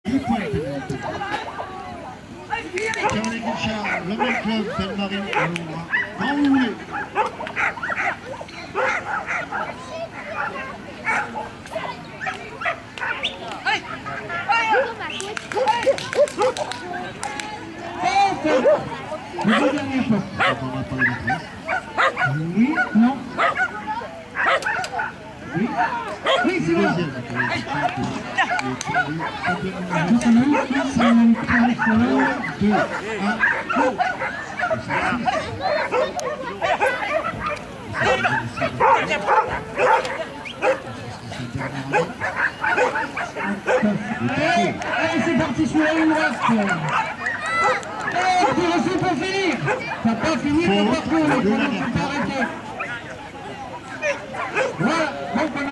Aya Jumping "plus" catégorie C Magny sur Tille juillet 2012
Concours d'agility, Magny sur Tille, 21 juillet 2012, Jumping "plus" catégorie C